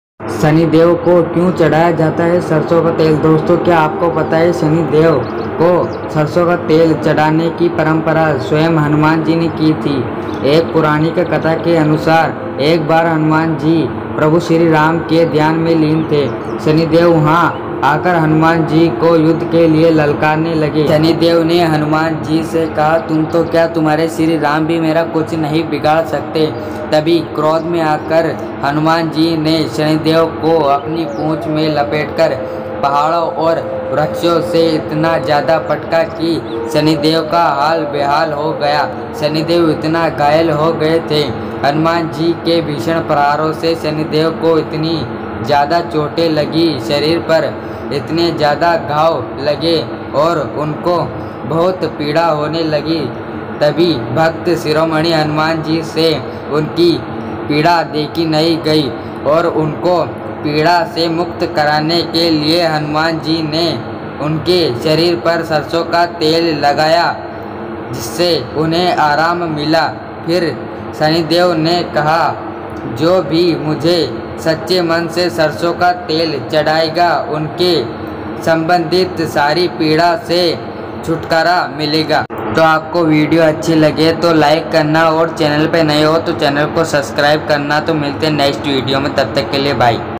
शनि देव को क्यों चढ़ाया जाता है सरसों का तेल🤔हनुमान जी ने क्यों कि शनि देव की पिटाई
Shani Dev ko kyon chadhaya jata hai sarson ka tel kya aapko pata hai 😱

Hanuman Ji ne kyon ki shanidev ki pitaai kis Karan

रावण सीता माता को कहां लेकर गया था
हरण करके